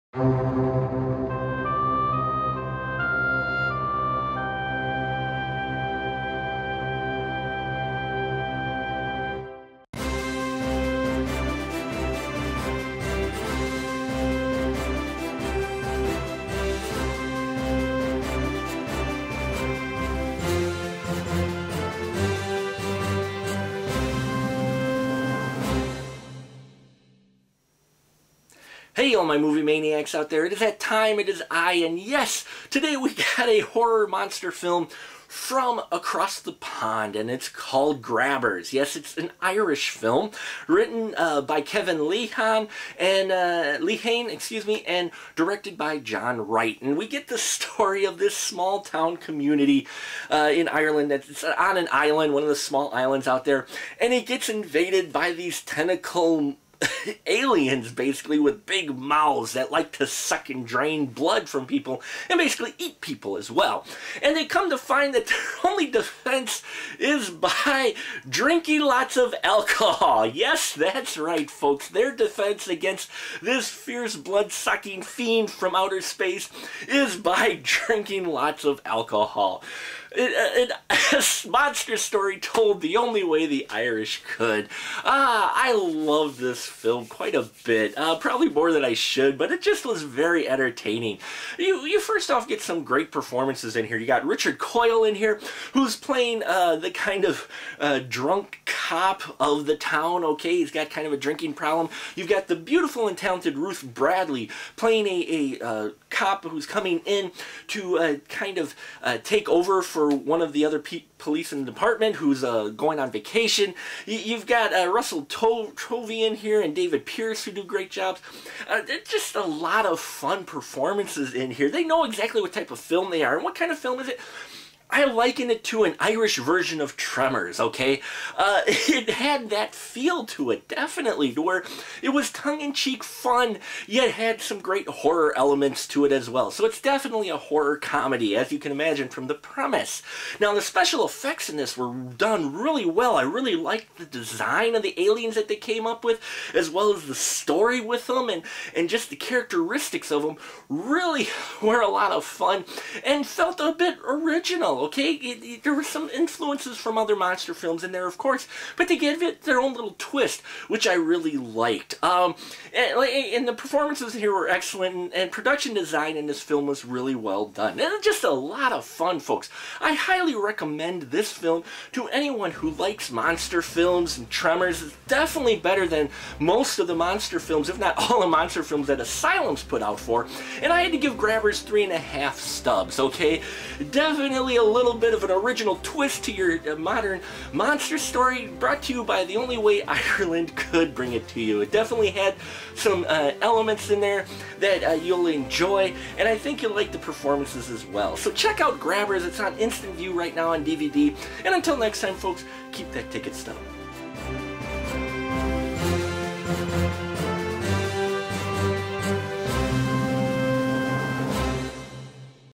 Grabbers (2012) Review on The Final Cut
Today we pull up to the bar and pour us a tall glass of a monster film from Ireland.